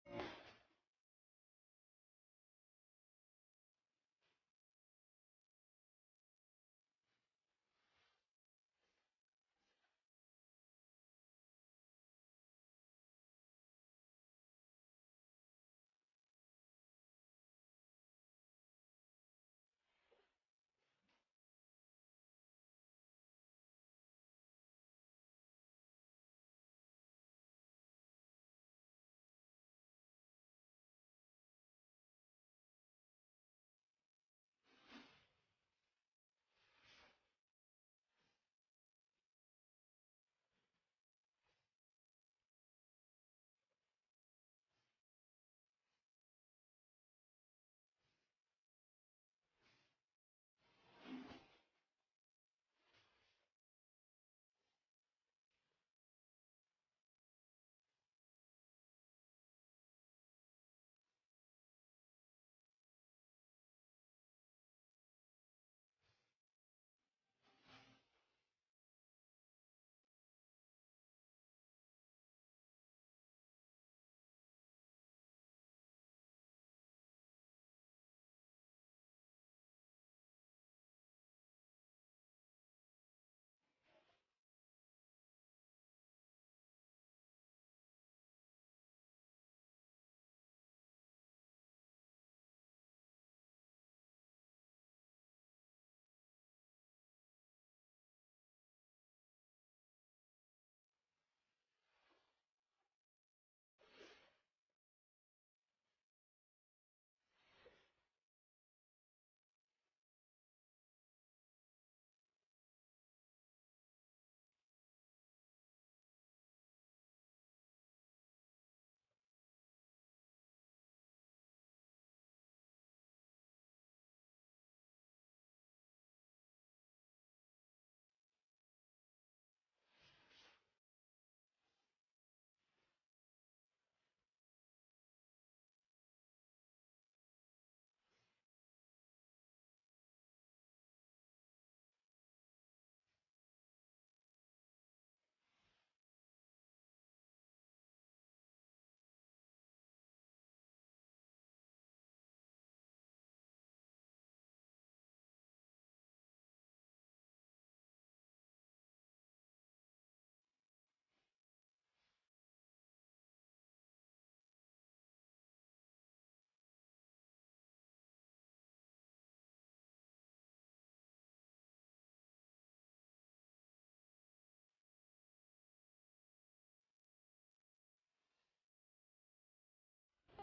SANA NASIL KIYDILAR 2 YAŞINDAKİ KIZ COCUGUNA TECAVÜZ ETTİ VE ÖLDÜRDÜ
SANA NASIL KIYDILAR BU MÜBAREK RAMAZAN AYINDA... :( ALLAH'IM ÇOCUKLARIMIZA KIYANLARI SEN KAHRU PERİŞAN ET...ÇOCUKLARIMIZ SANA EMANET